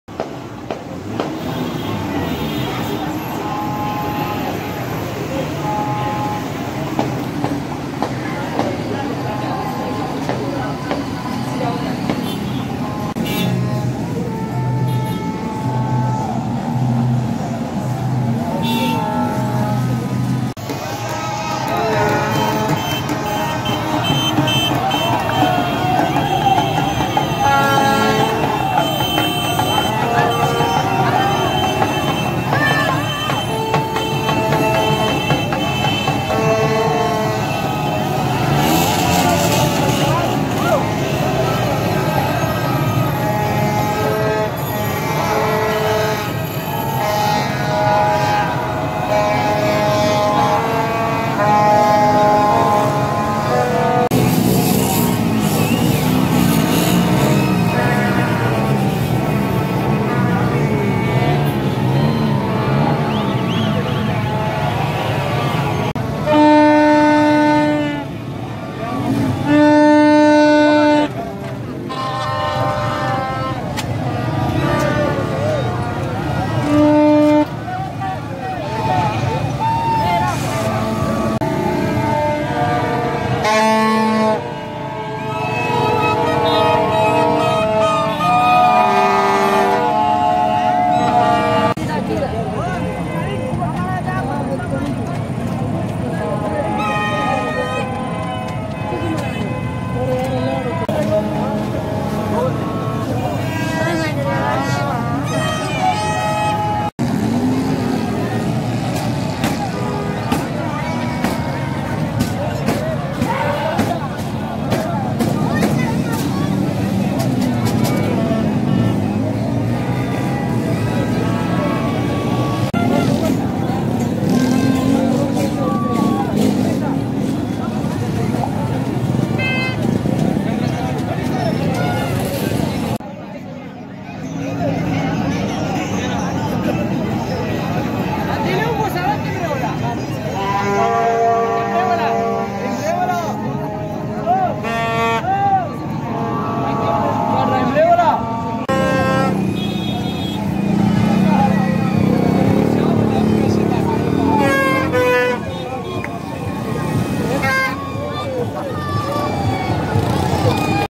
Jashn-e-Azadi 14th August 2022 Celebrations at Phase-3 Chowk Peshawar
75th anniversary of Pakistan is celebrating at Peshawar Phase-3 chowk. The 14th August will be celebrated tomorrow insha Allah.